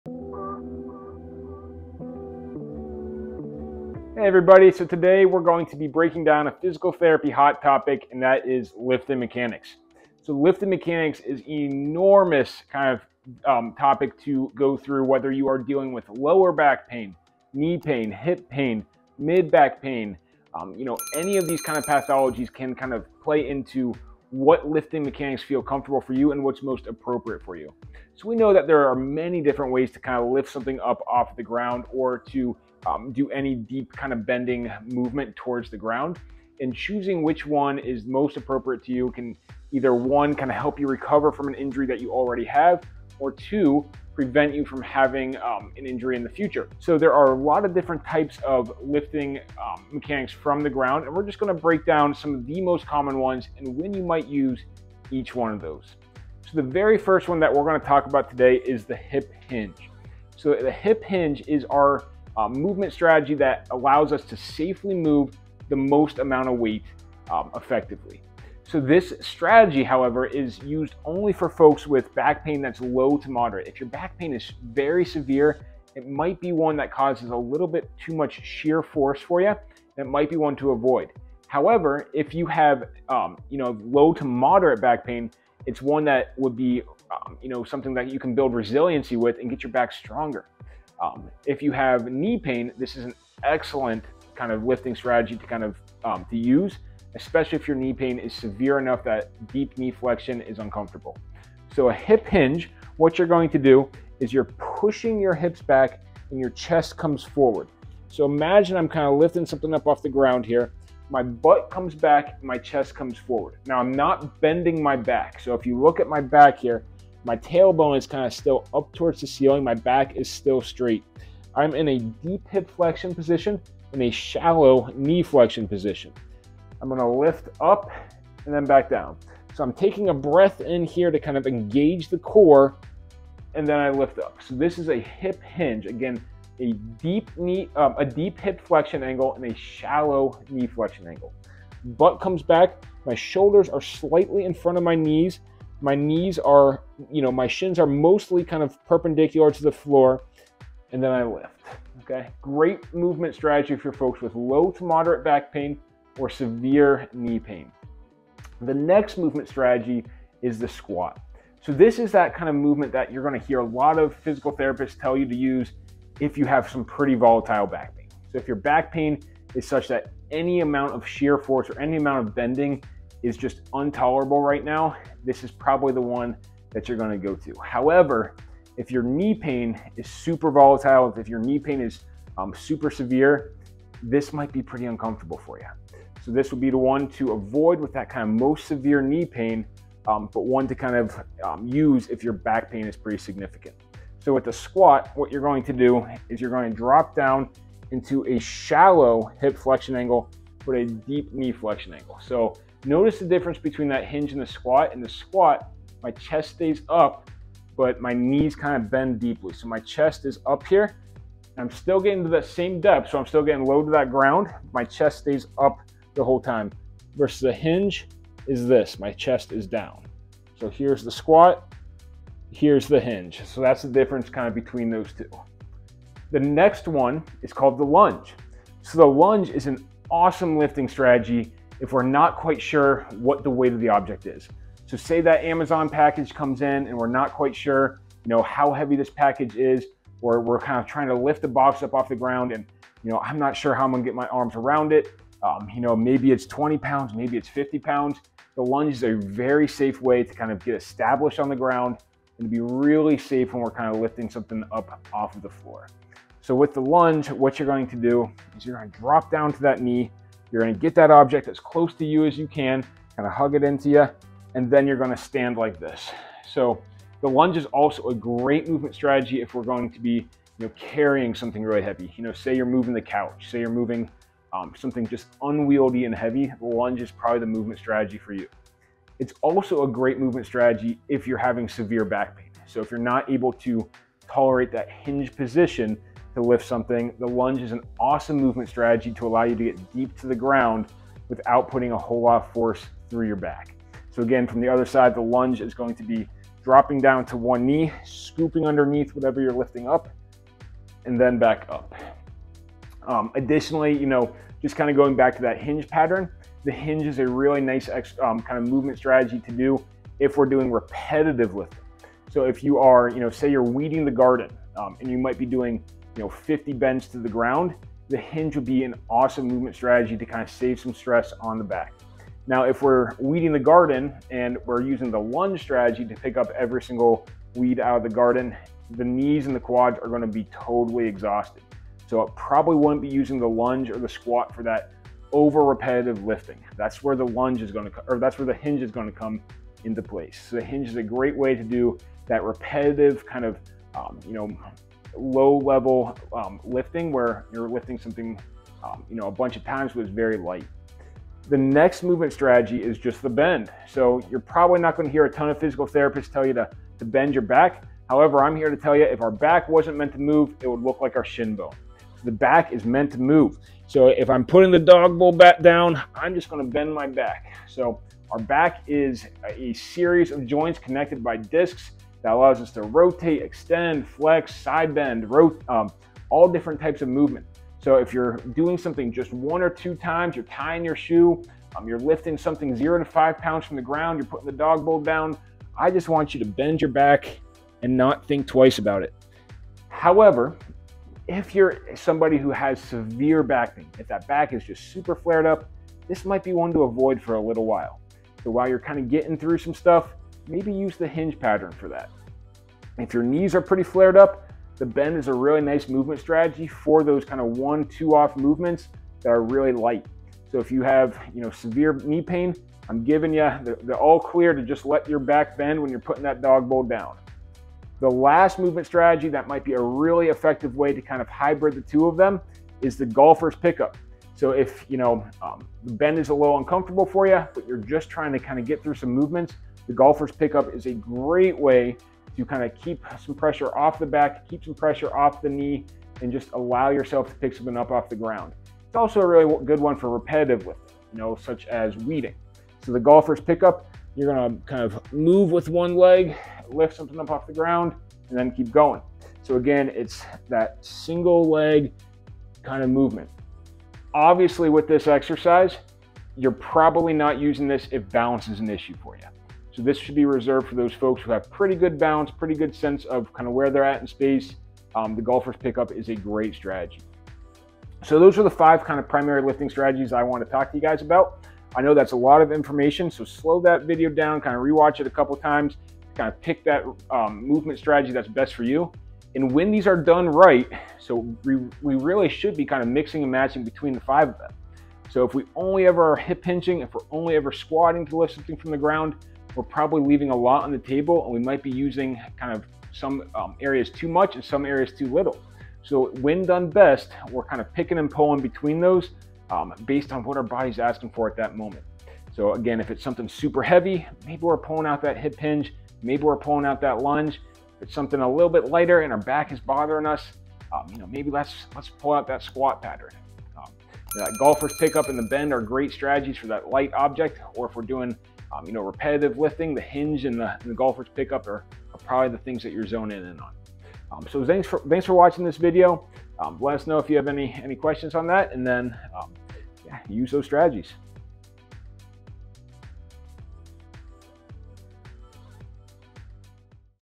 💪 Master Safe Lifting: Pro Techniques for Injury-Proof Lifts
Unlock the secrets to injury-free lifting with PT Dan Patella's expert guidance!

In this must-watch video, learn essential techniques for mastering the hip hinge, squat, lunge, bend, and golfer's pickup.

Safeguard your back and boost your strength with Dan's pro tips. Don't miss out—watch now!